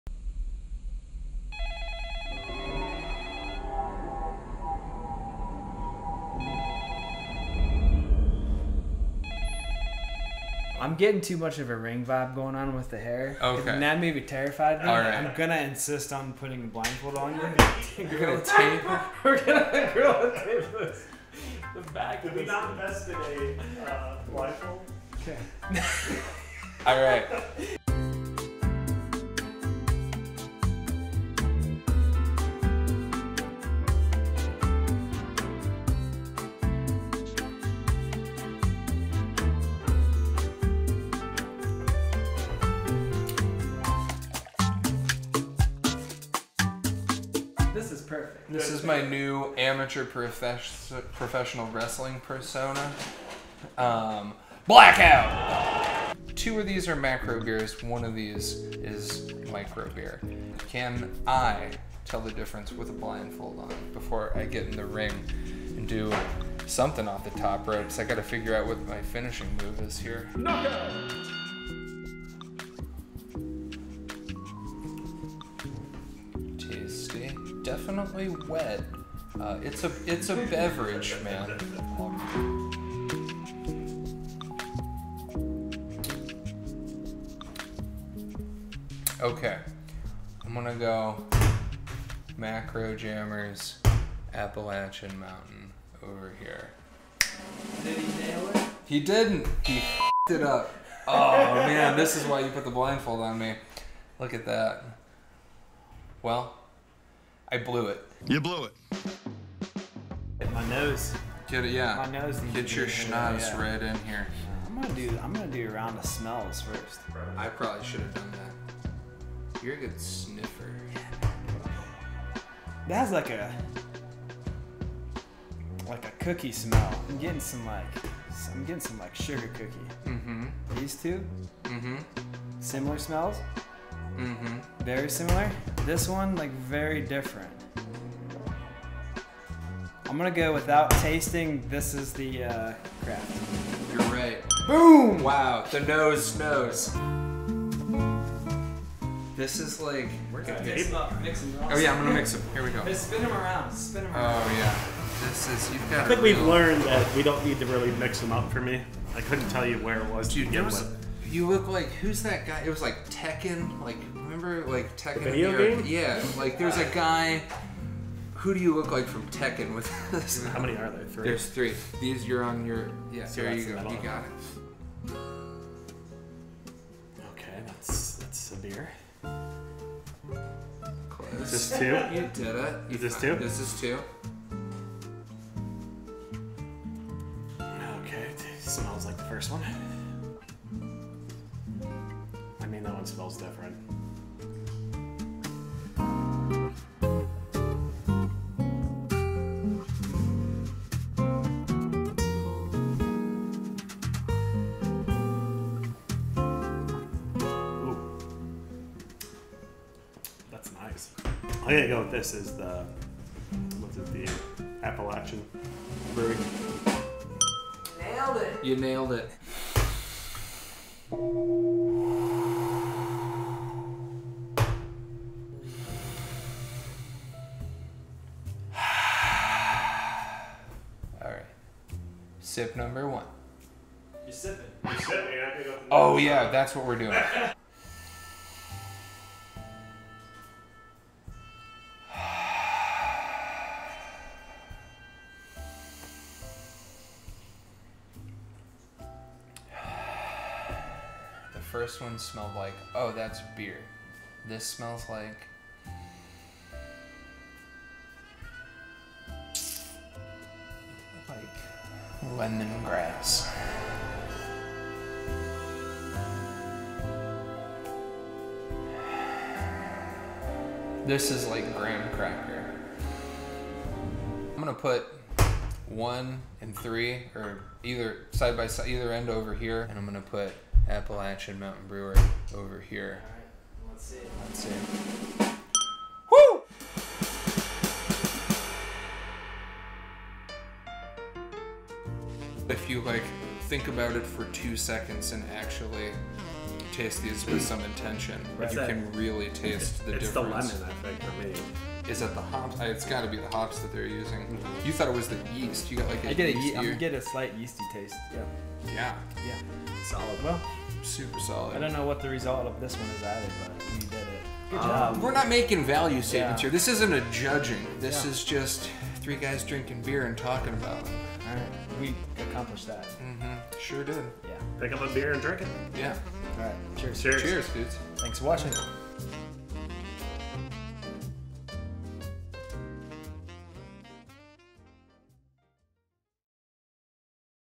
BeerBusters E2: Macro Vs. Micro Lager Challenge
In this comedy show about beer we do an experiment to bust a myth and see if there's really any difference between commercial beer and craft beer. Can people really taste the difference between craft beer and commercial beer? By disguising beers and doing a blind taste test we find out in this video. This video is for people who enjoy Mythbusters, science experiments, comedy shows, videos from the channel "Cut", and are also interested in the craft and commercial beer industry.